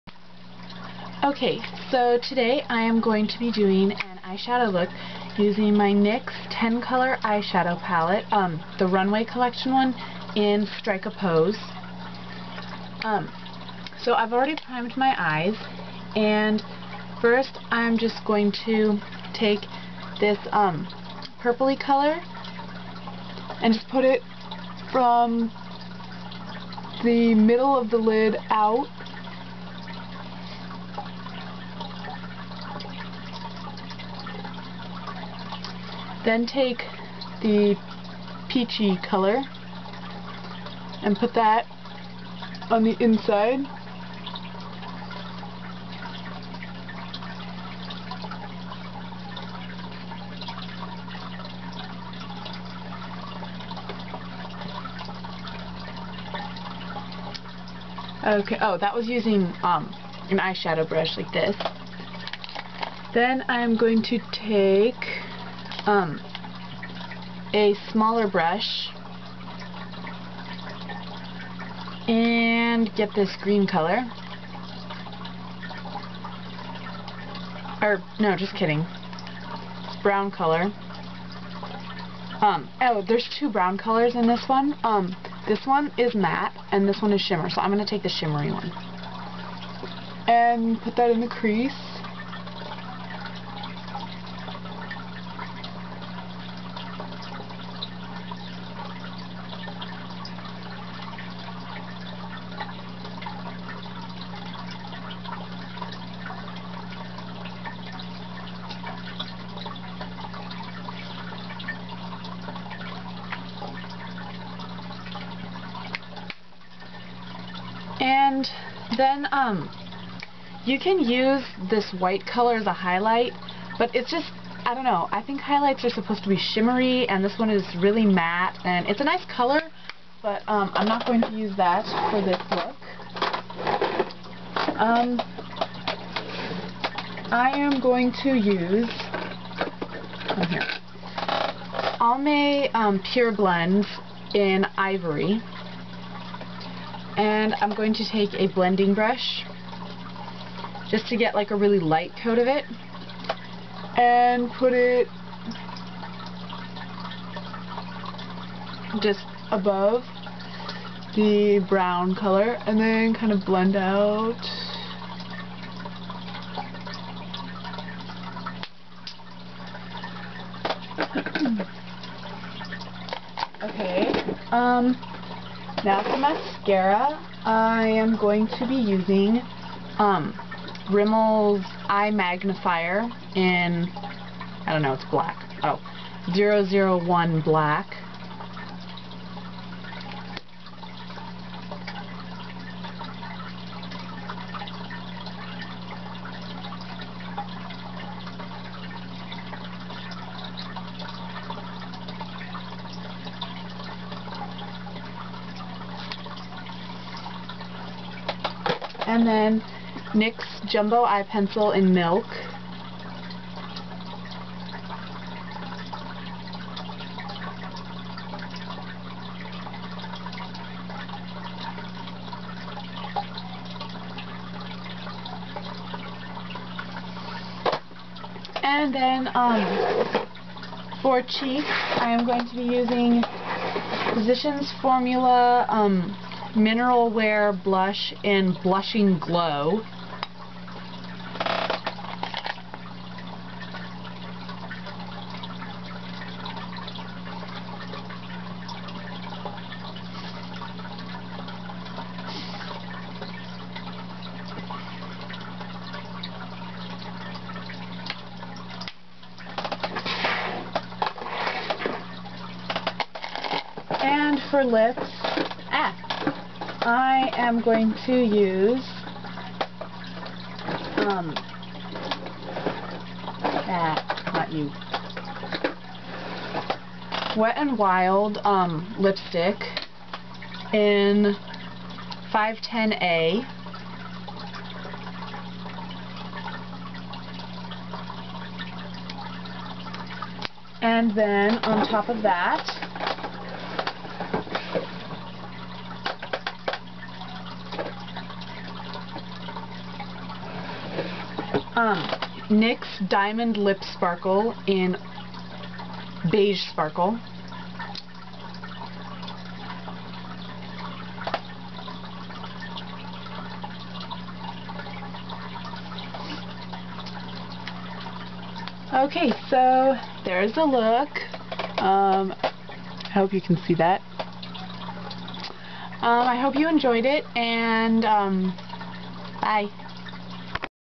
Peach and Purple Look using NYX Palette
This look was done using the NYX 10 color palette from the Runway Collection, in Strike A Pose, NYX Jumbo Eye Pencil in Milk, Rimmel Eye Magnifier mascara in Black, Wet N Wild lipstick in 510A, under NYX Diamond Sparkle lip gloss in Beige Sparkle, and Physician's Formula Mineral Wear blush in Blushing Glow.